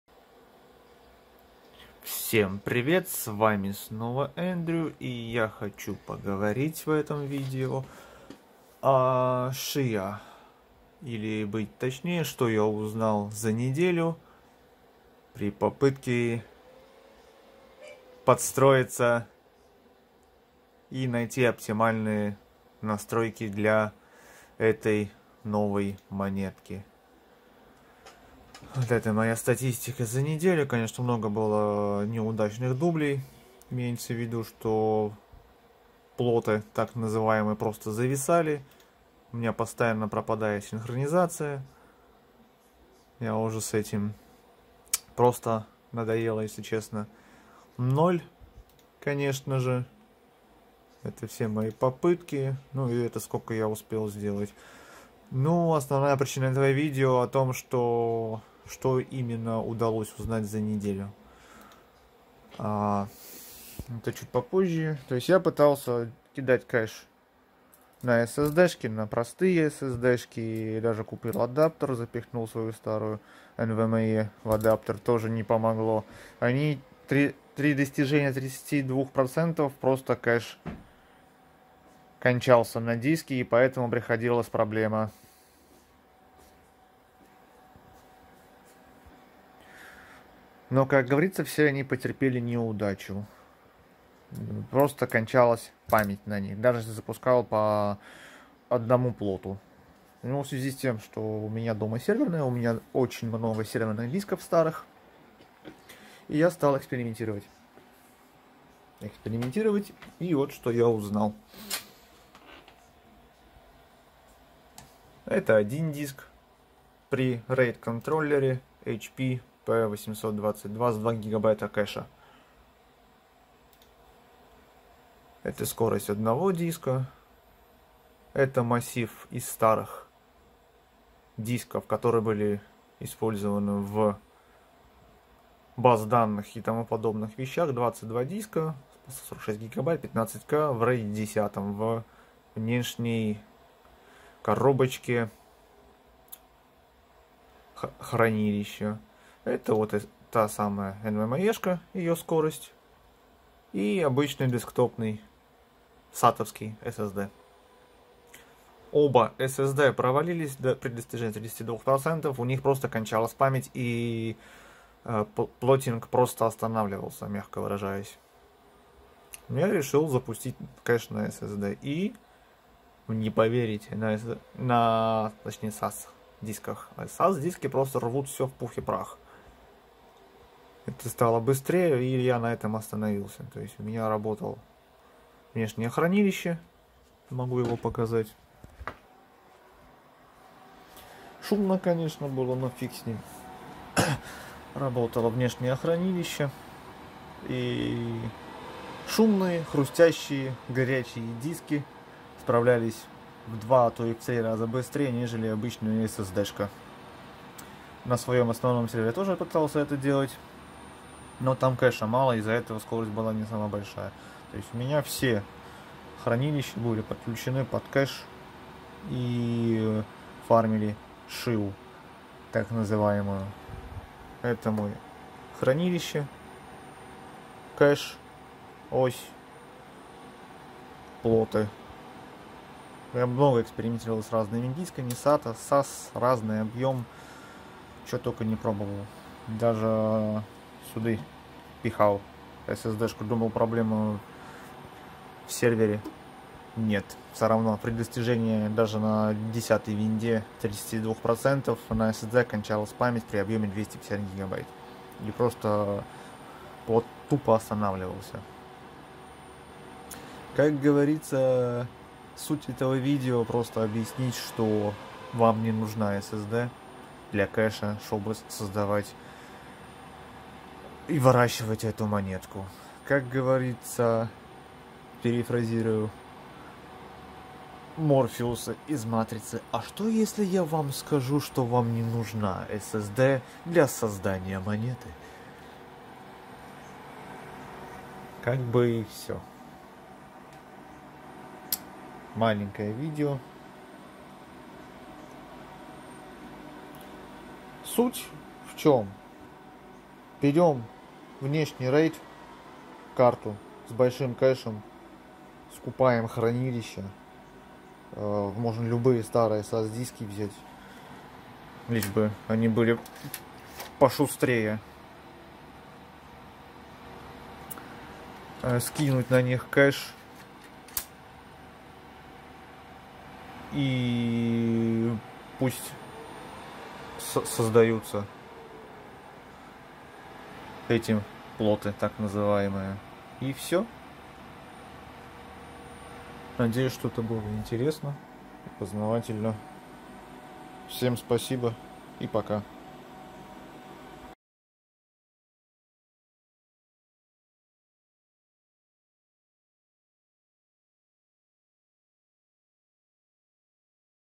RUS: Фарм Chia за неделю, что узнал
Всем привет!
В этом видео постараюсь объяснить про Chia что узнал за неделю.
Версия клиента - 1.1.5

Вуаля!